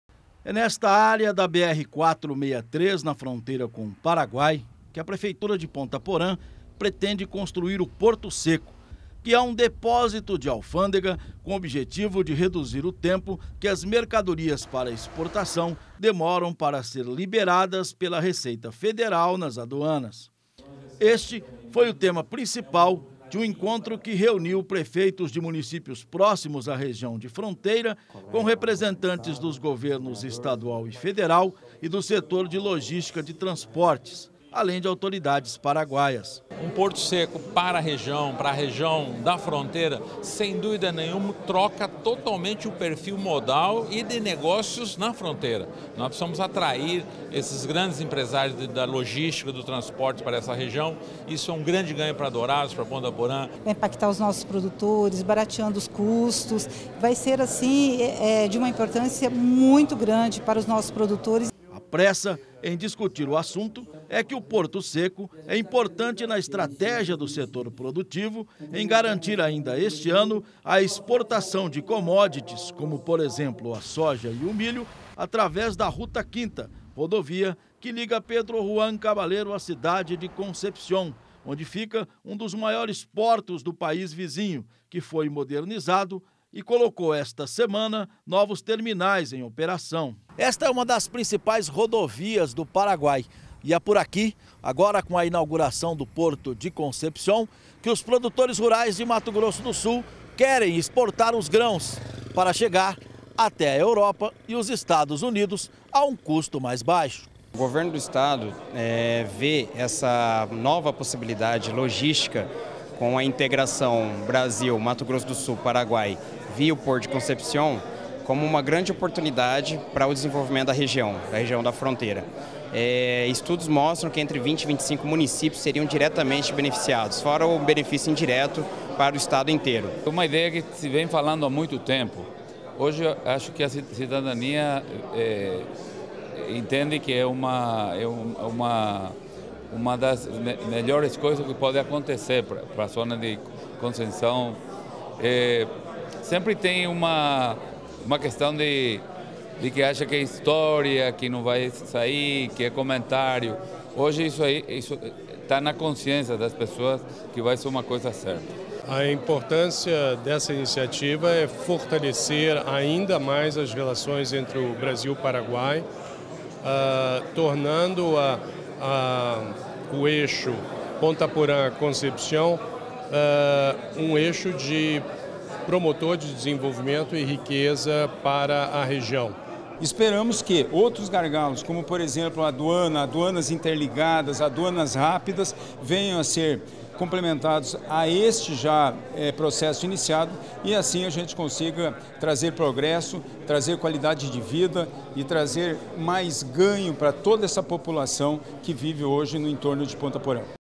Autoridades debatem criação de Porto Seco para agilizar exportação de grãos
Autoridades brasileiras e paraguaias se reuniram para discutir a criação do Porto Seco de Ponta Porã. A ideia é criar um corredor para o escoamento da produção do estado até o Oceano Pacífico, passando pelo país vizinho.